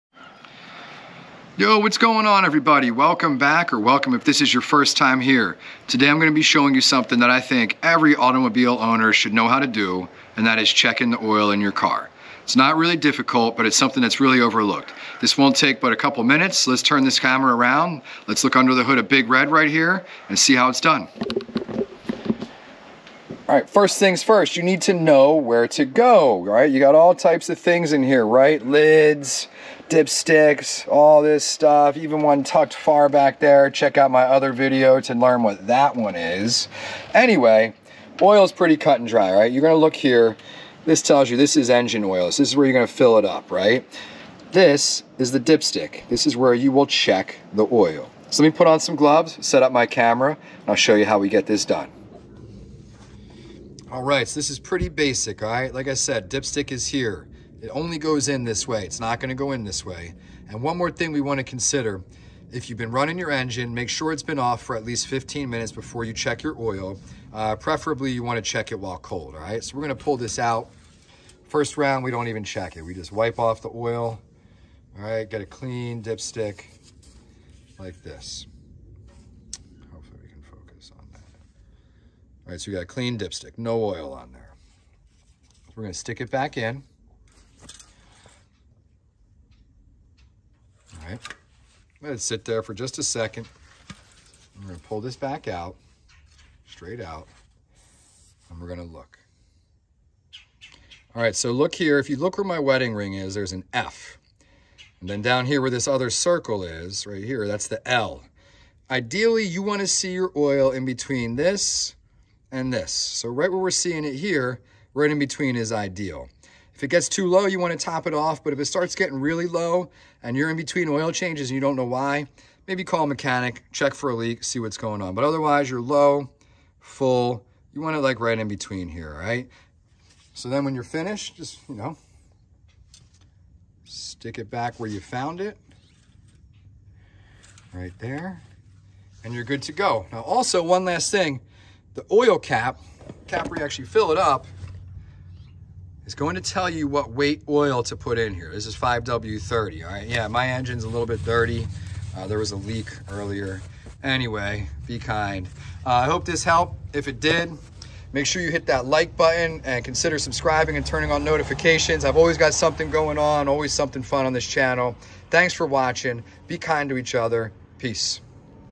2010 Subaru Forester (3rd Gen) - How to check your oil
In this tutorial, I show you how to check the oil in a 2010 Subaru Forester.
This is definitely something every car owner should know how to do, as not having enough oil in your engine can cause serious damage, such as seizing up your engine.
Follow along and learn how to do this for yourself.
Also, this will work on any car.